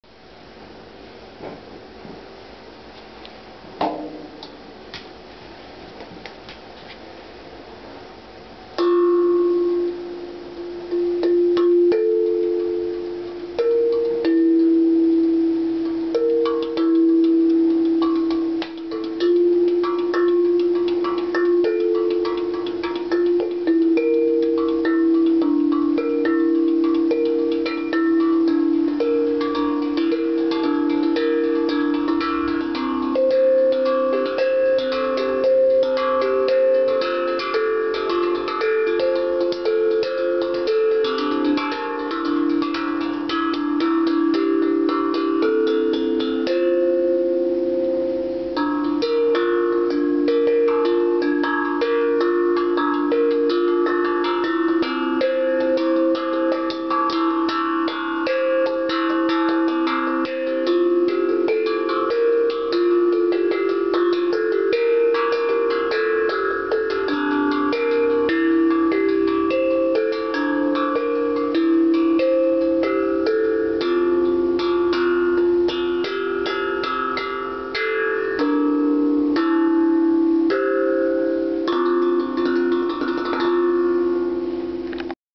steel tongue drum by rock creek steel drums
still trying to get the "hang" of this thing...not bad for only playing it a few days! as a drummer who loves to explore melodies, I recommend a steel tongue drum as a cheap alternative to the elusive hang drum...this one is made by a company called rock creek steel drums and I added the foam bottom to deepen the resonance of the steel shell, which is actually a regular propane tank you'd find on your grill with a few modifications. Special thanks to Kevin Martin at rock creek steel drums!